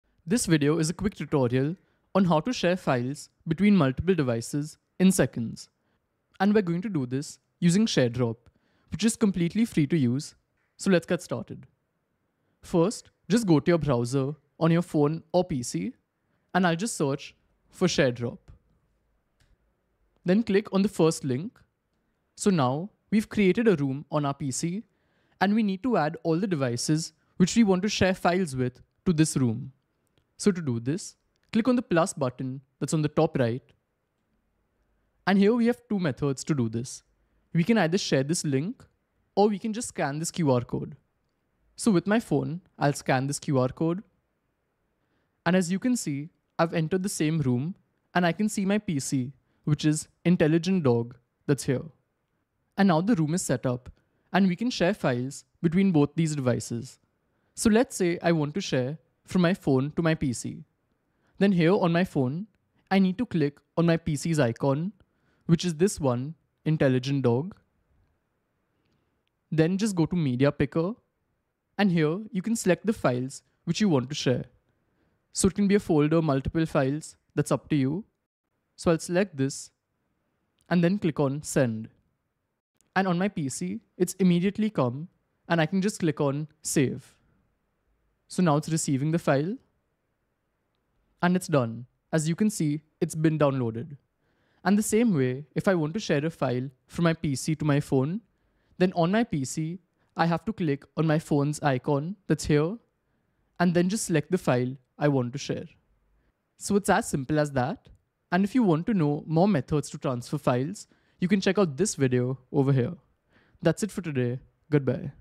How To Transfer Files From Mobile to PC (in Seconds)
Seamless File Transfer: Phone to PC in Seconds with Sharedrop

In our latest tutorial, we'll show you how to effortlessly transfer files between your phone and PC wirelessly in seconds using Sharedrop. From photos to documents, experience the magic of quick and convenient file sharing without the need for USB cables or cloud services.

What You'll Learn:
Introduction to Sharedrop: Get to know the intuitive and user-friendly Sharedrop platform for seamless wireless file transfers.

Connecting Phone and PC: Follow our step-by-step guide on how to connect your phone and PC on Sharedrop, ensuring a stable and secure connection.

Transferring Files: Experience the speed of transferring files in seconds, whether it's photos, videos, or documents, directly between your devices.

eal-Time Interaction: Explore the real-time interaction feature of Sharedrop, allowing you to see files being sent and received instantly.

🌟 Benefits of Wireless File Transfer:
Cable-Free Convenience: Enjoy the freedom of wireless file transfer without the need for USB cables.
Quick and Efficient: Transfer files in seconds, saving you time and effort.
Multi-Platform Compatibility: Sharedrop works seamlessly across various devices, ensuring compatibility with your phone and PC.

Timestamps:
00:00 - Introduction
00:23 - Add Devices
00:53 - Share Files